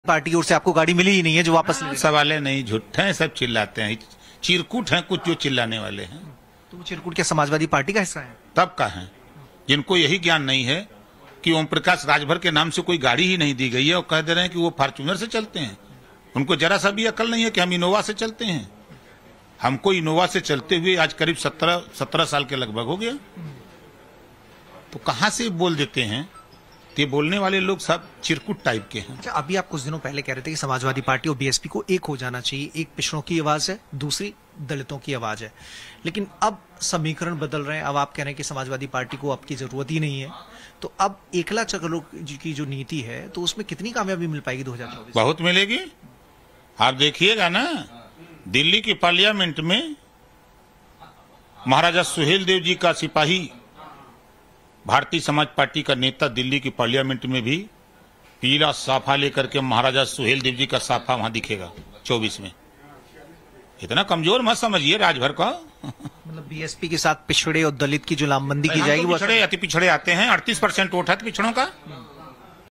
'फॉर्चूनर से चलने वाले' बयान पर भड़के OP Rajbhar बोले 'ये सब चिरकुट टाइप नेता हैं'
'ये सब चिरकुट टाइप नेता हैं हम इन्नोवा से सफर करते हैं', जब 'फॉर्चूनर से चलने वाले' बयान पर भड़के OP Rajbhar | Times Now Navbharat से बोले राजभर